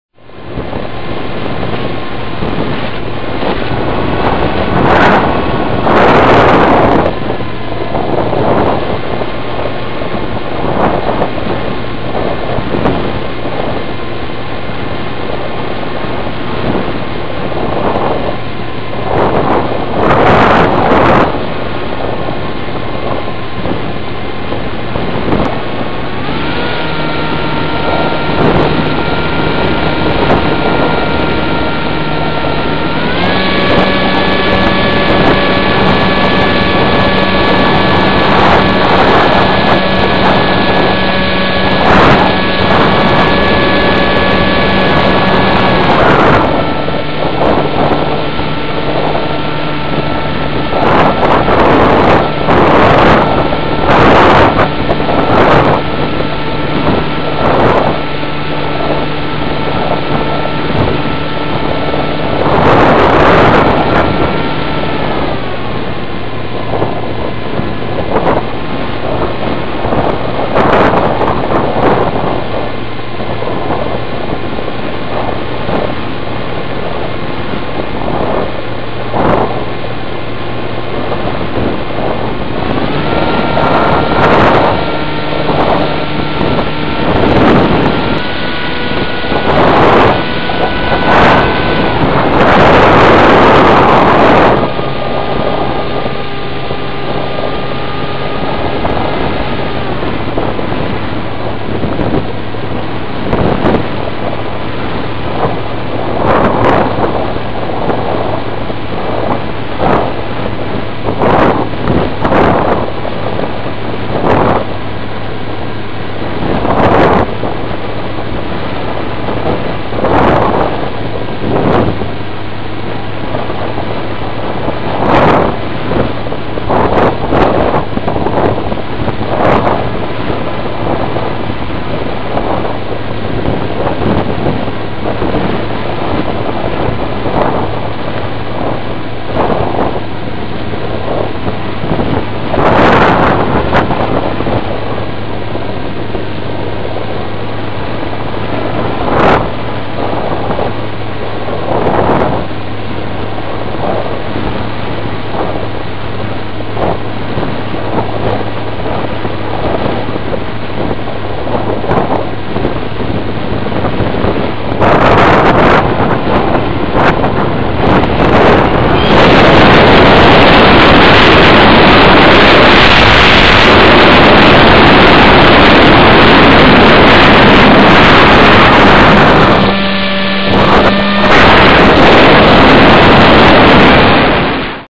RC Easy Star, FlycamOne, San Marcos, Discovery
Discovery Lake in the distance.  Rebuilt plane working well.